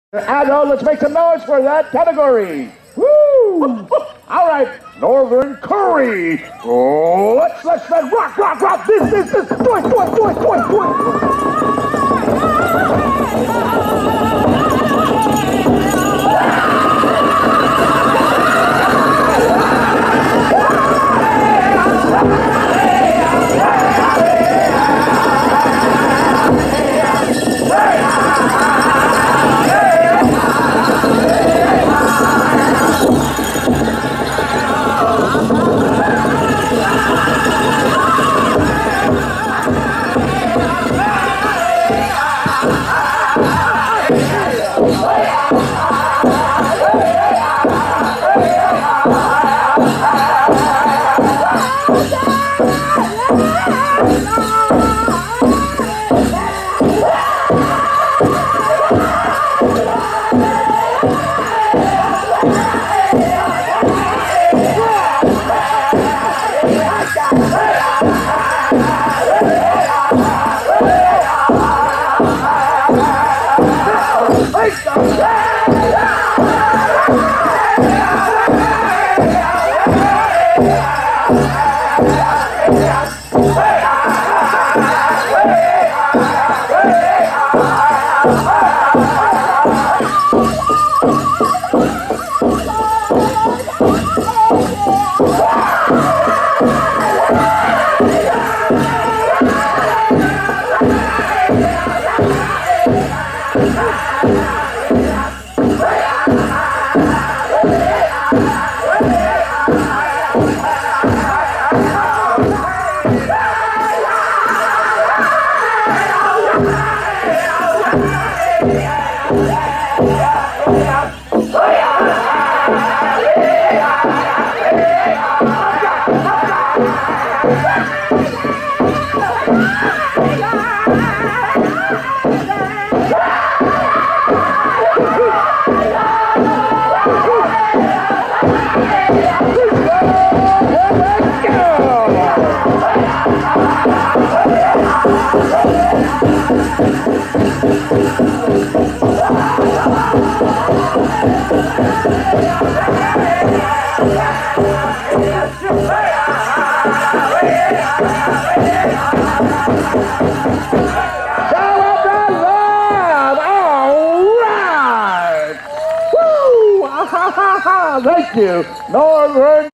N. CREE @ Red Mtn Mens Chicken SNEAK UP
SNEAK UP Song for MENS PRAIRIE CHICKEN - NORTHERN CREE @ 2014 Red Mountain Eagle Powwow Scottsdale AZ